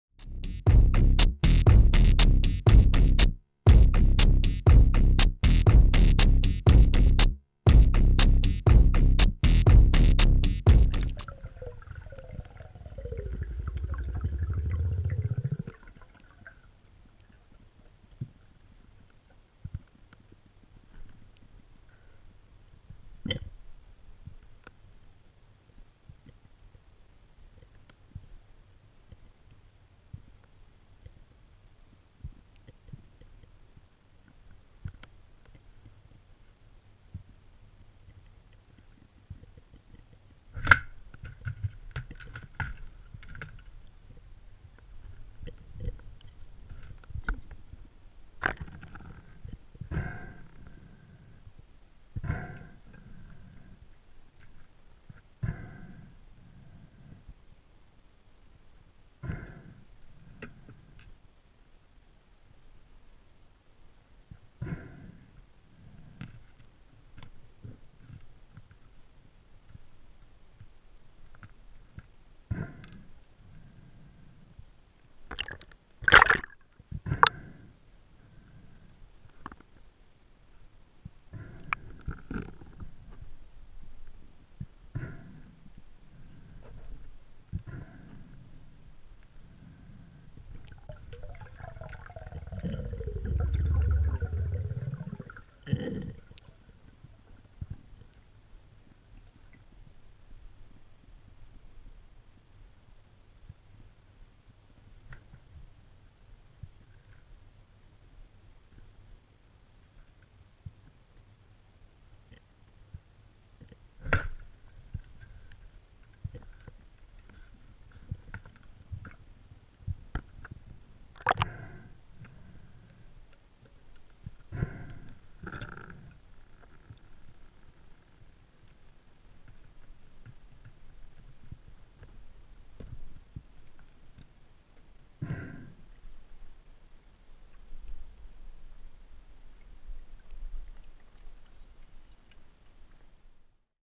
404 How Not To Use a Speargun - Fishing Gear Abuse
Spearfishing freshwater perch with a pole spear. The spear hit a rock, but luckily the fish was stoned by the paralyzer tip. I used my speargun as a fish stringer, not quite normal.